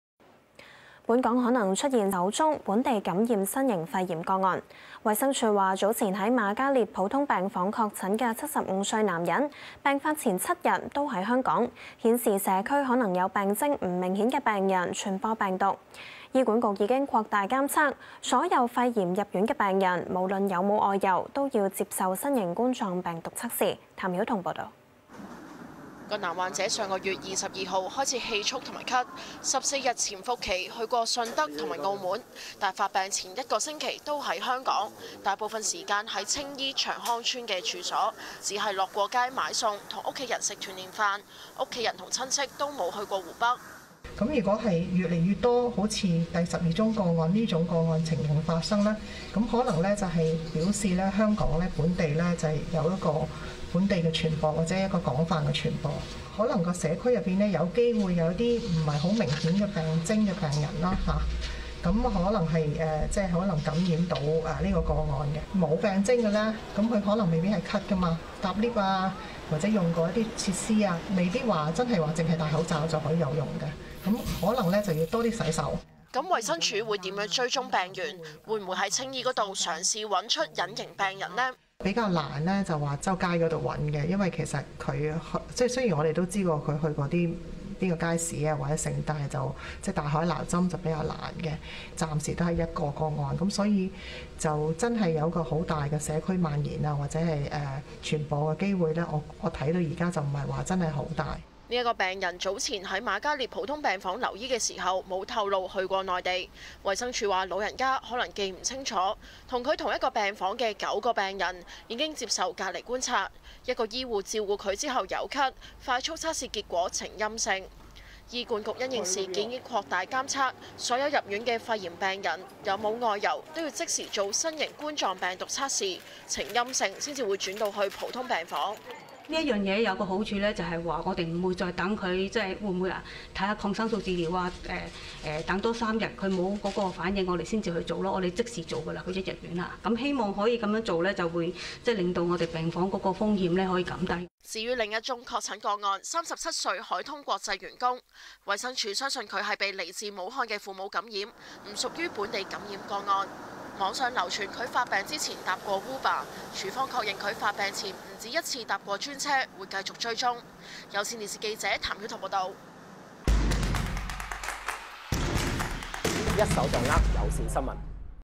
武漢肺炎 - 青衣75歲確診病人 或成首宗本地感染個案;衞生署：社區或有病徵不明顯病人傳播病毒 - 20200201 - 香港新聞 - 有線新聞 i-Cable News
【武漢肺炎】
【青衣75歲確診病人　或成首宗本地感染個案】
【衞生署：社區或有病徵不明顯病人傳播病毒】

本港可能出現首宗，本地感染新型肺炎個案。衞生署指早前在瑪嘉烈普通病房確診的75歲男人，病發前7日都在香港，不排除是本地感染，顯示社區或有病徵不明顯的病人傳播病毒。

醫管局已擴大監測，所有肺炎入院病人，不論有否外遊，都要接受新型冠狀病毒測試。